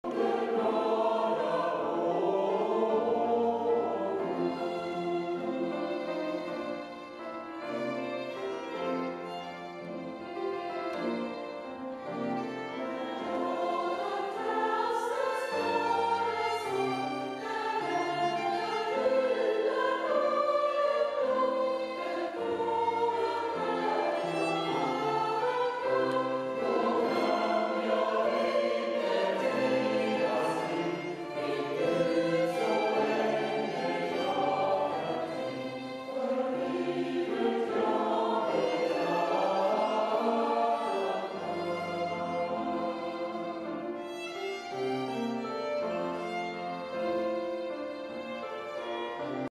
Tensta, Sweden- Church Choir during Zorn week 2010
Tensta, Sweden church choir. Choir composed from Tensta and Lena churches combined. Zorn week June 2010.